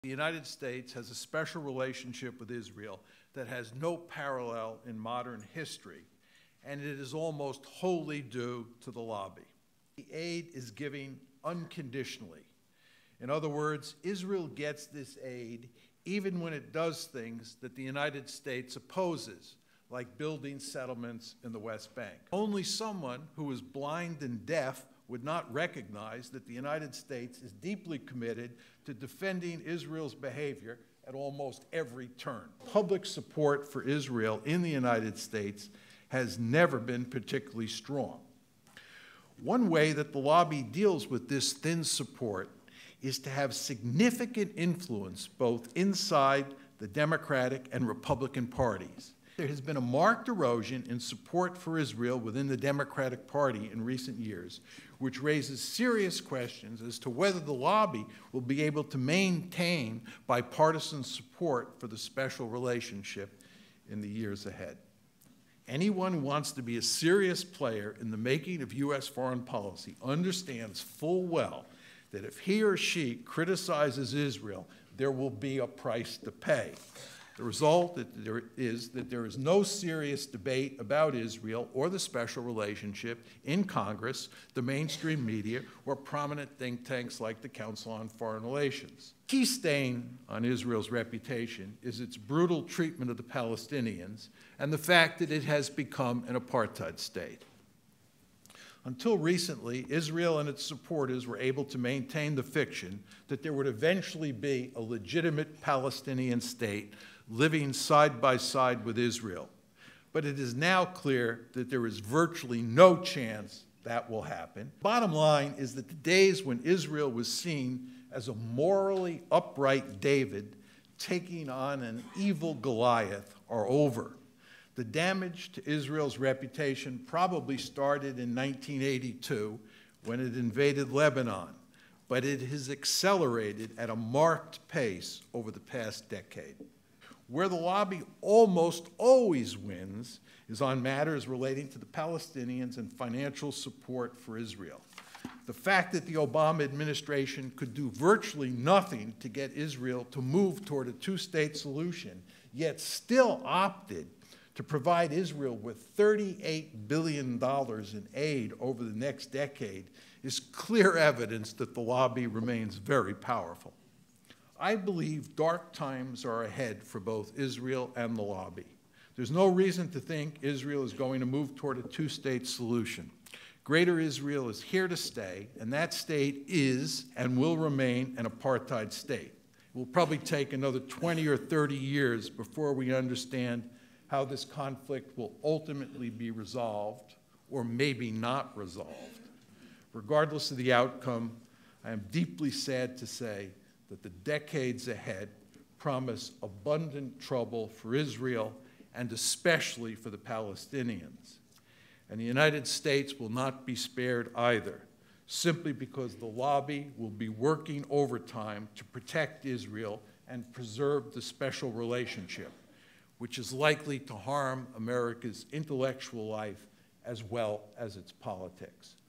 John Mearsheimer: Dark Times Ahead for Israel
Highlights from John Mearsheimer's keynote address at the March 24, 2017 Israel Lobby and American Policy confernece.